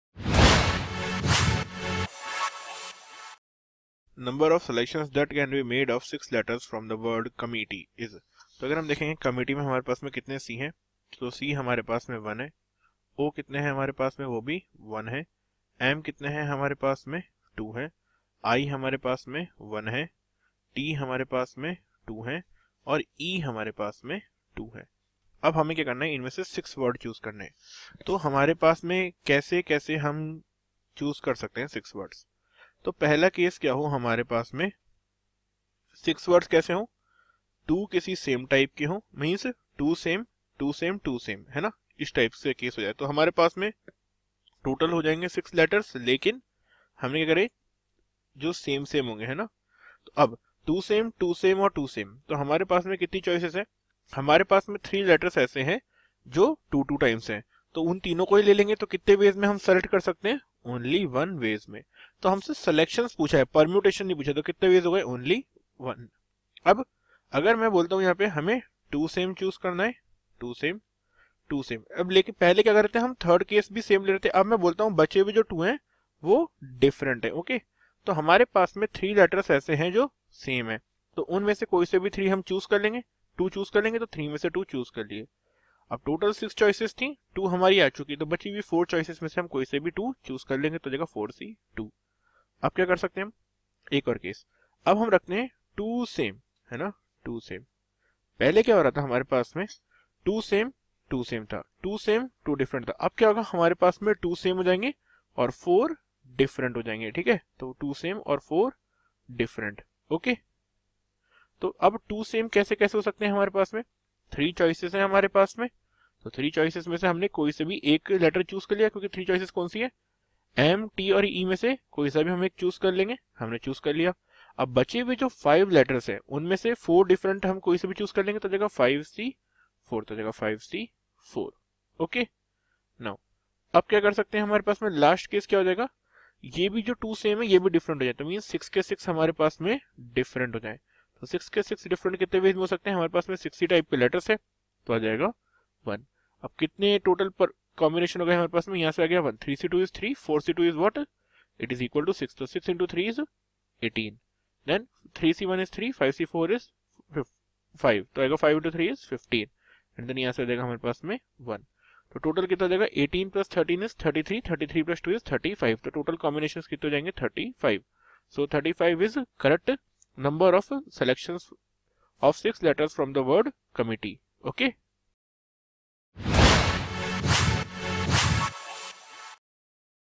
Number of selections that can be made of 6 letters word from "COMMITTEE" is
To ask Unlimited Maths doubts download Doubtnut Number of selections that can be made of 6 letters word from "COMMITTEE" is